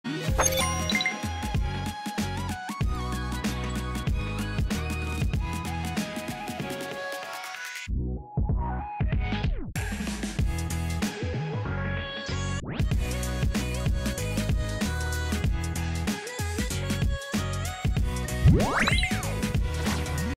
Little Showcase! - "Sophie Neuenmuller: Everyday Me Ver." Scale Figure by KT model+ (& Good Smile)
(Atelier Sophie: The Alchemist of the Mysterious Book)